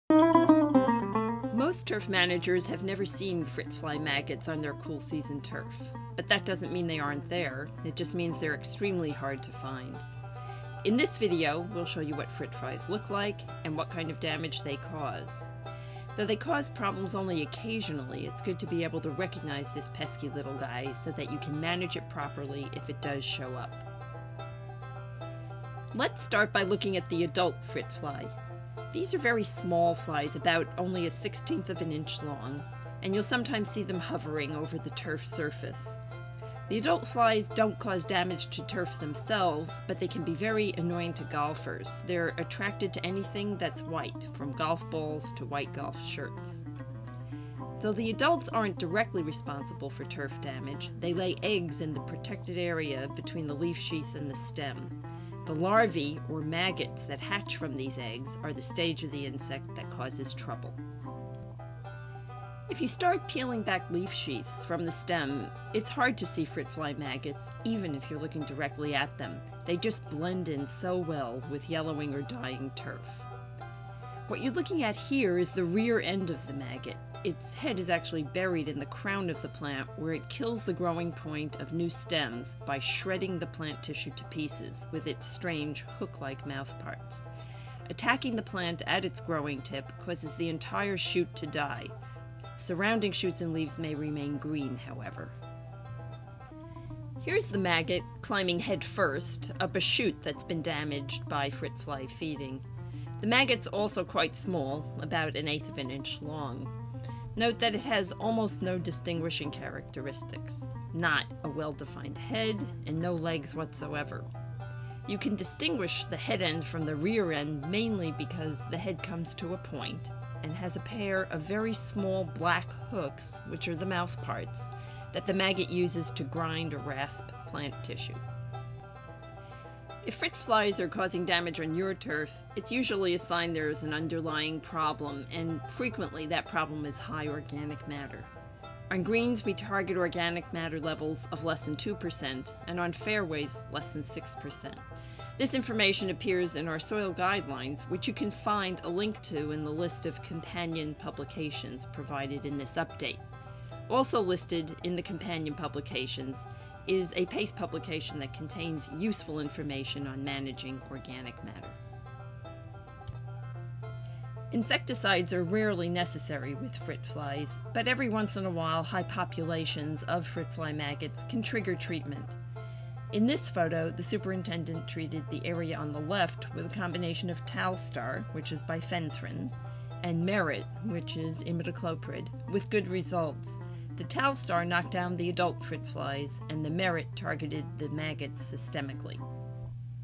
Frit flies
This video describes frit flies and their damage.